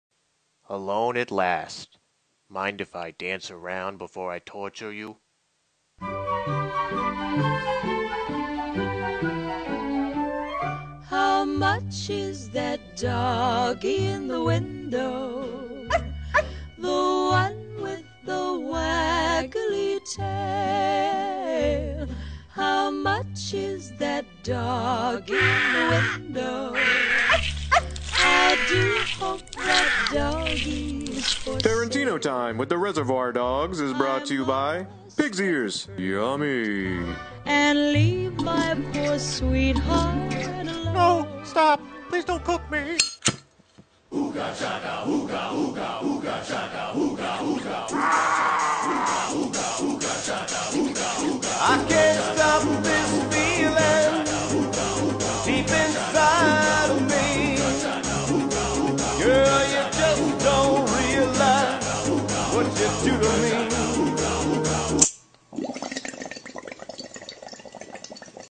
If Quentin Tarantino did a Kids Show
A Scene from 'Tarantino Time with the Reservoir Dogs'. Shot & edited by Keith Krass. A Lenzcrack Production